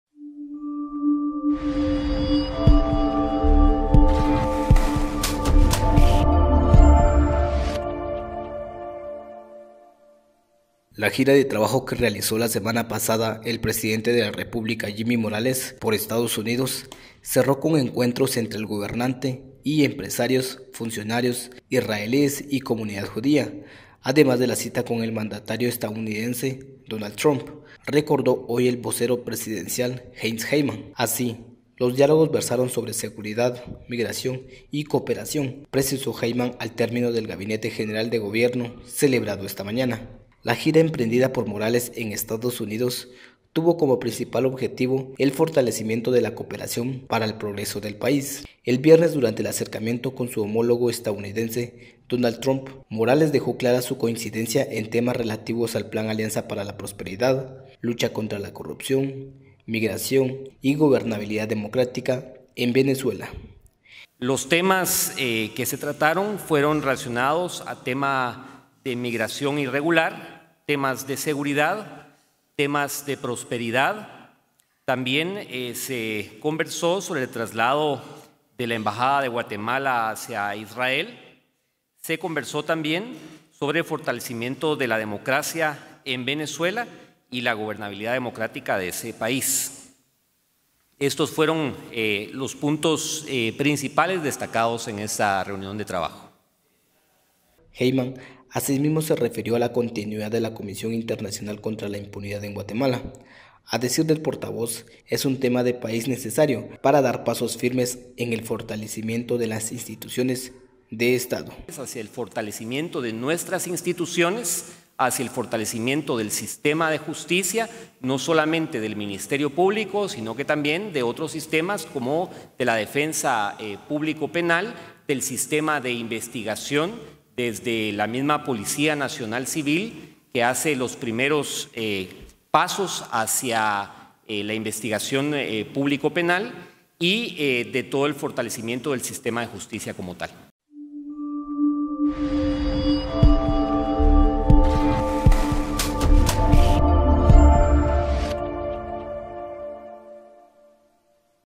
Gira del presidente cierra con diálogos sobre seguridad, migración y cooperación
La gira de trabajo que realizó la semana pasada el presidente de la República, Jimmy Morales, por Estados Unidos, cerró con encuentros entre el gobernante y empresarios, funcionarios israelíes y comunidad judía, además de la cita con el mandatario estadounidense Donald Trump, recordó hoy el vocero presidencial Heinz Hiemann.

Así, los diálogos versaron sobre seguridad, migración y cooperación, precisó Hiemann al término del Gabinete General de Gobierno celebrado esta mañana.

La gira emprendida por Morales en EE. UU. Tuvo como principal objetivo el fortalecimiento de la cooperación para el progreso del país.

El viernes, durante el acercamiento con su homologo estadounidense, Donald Trump, Morales dejó clara su coincidencia en temas relativos al Plan Alianza para la Prosperidad, lucha contra la corrupción, migración y gobernabilidad democrática en Venezuela.

continuidad de la CICIG

Hiemann, asimismo, se refirió a la continuidad de la Comisión Internacional Contra la Impunidad en Guatemala (Cicig). A decir del portavoz, es un tema de país necesario para dar pasos firmes en el fortalecimiento de las instituciones de Estado.